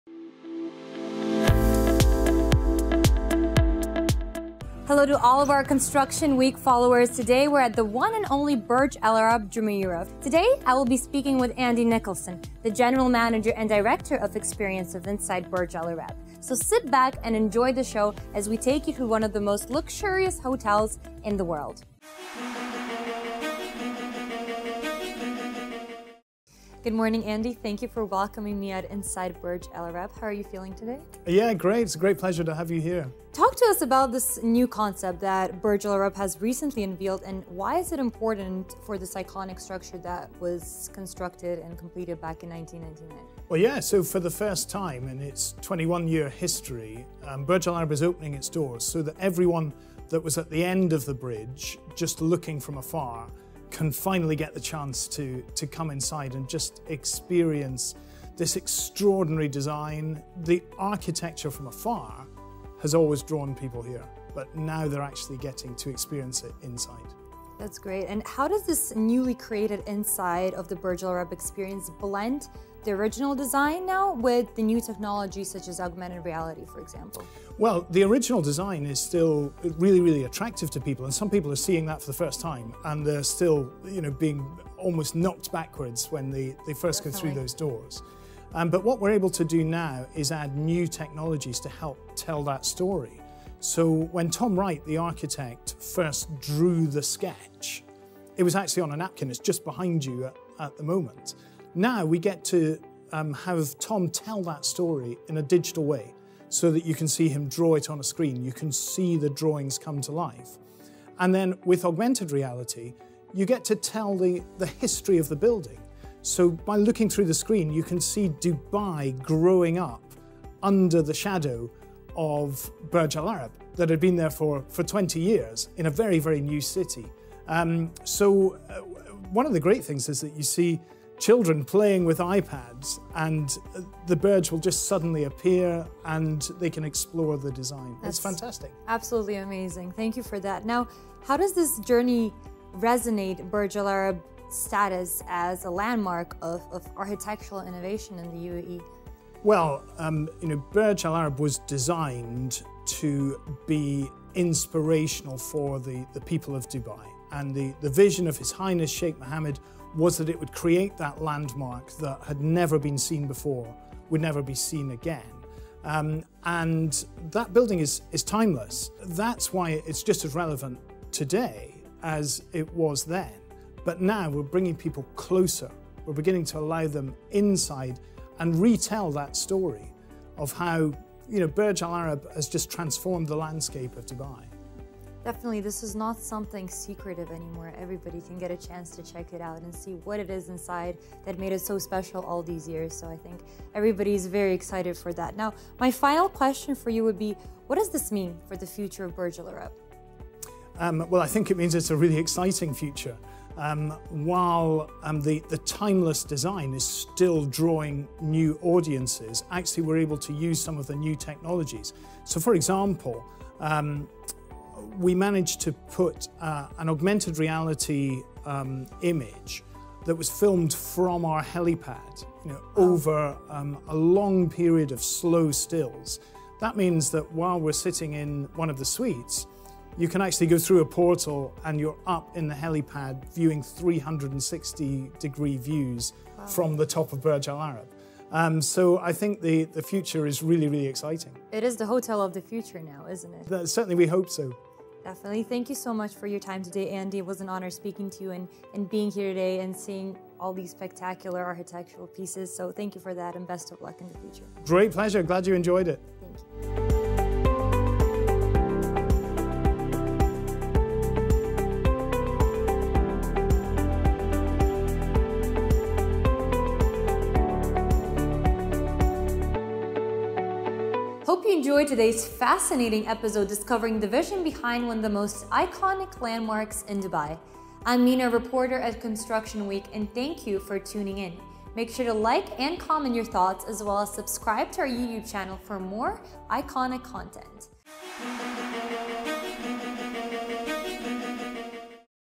Tracing the design and construction journey of Burj Al Arab
As Construction Week Middle East launches the Icons Deconstructed series, we get a peek inside the marvelous Burj Al Arab, where Andy Nicholson, General Manager & Director of Experience at Burj Al Arab takes us through the design and architecture journey of the iconic landmark that was built in 1999.

Designed by the multidisciplinary consultancy Atkins, led by architect Tom Wright, the Burj Al Arab was built to resemble the billowing spinnaker sail of a J-class yacht. Its lavish interiors are the work of interior designer Khuan Chew, whose firm, KCA International, was then headquartered in London.

PRODUCER
Mina Vucic

VIDEOGRAPHERS
Muhammed Abugosh and Muhammad Kaleem

SOUND ENGINEER
Borys Hridin

PHOTOGRAPHER
Efraim Evidor